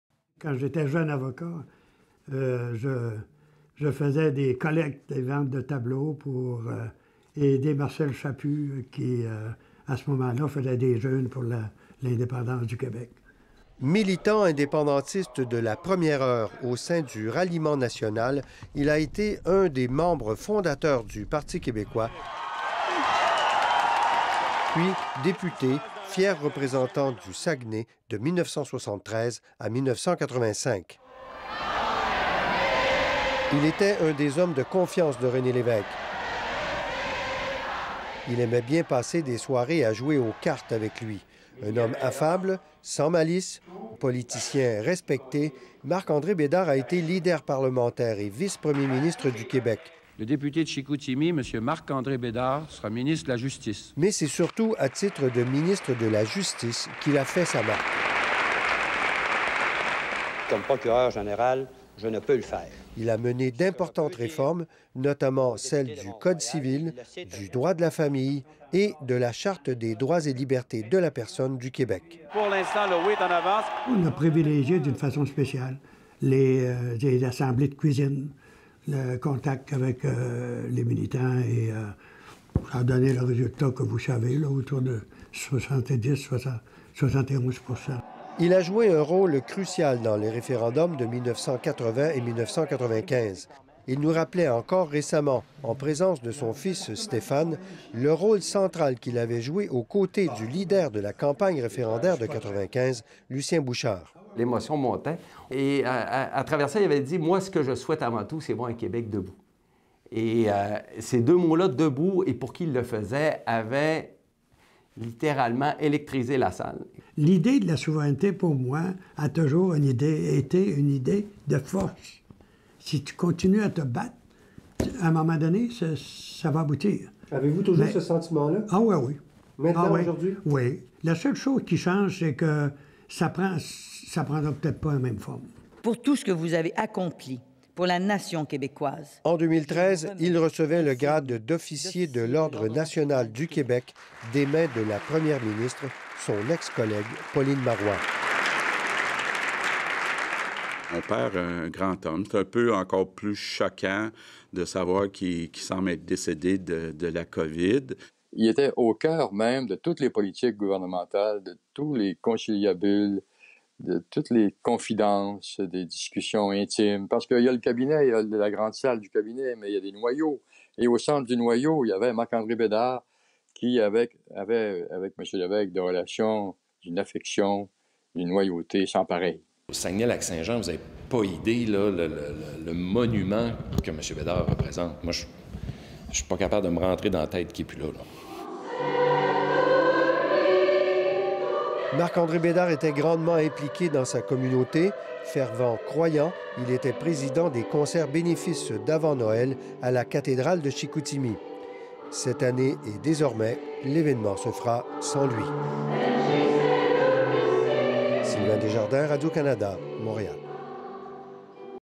Décès de l’ex-ministre Marc-André Bédard
Le Québec a perdu l'un des grands ministres de son histoire. Marc-André Bédard a été emporté par la COVID-19 à 85 ans. Membre fondateur du Parti québécois et proche collaborateur de René Lévesque, l'ancien député de Chicoutimi a joué un rôle de premier plan sur la scène politique québécoise.  Sylvain Desjardins l'avait rencontré pour un reportage, il y a quelques semaines seulement. Il nous dresse le portrait de ce réformateur et fier indépendantiste.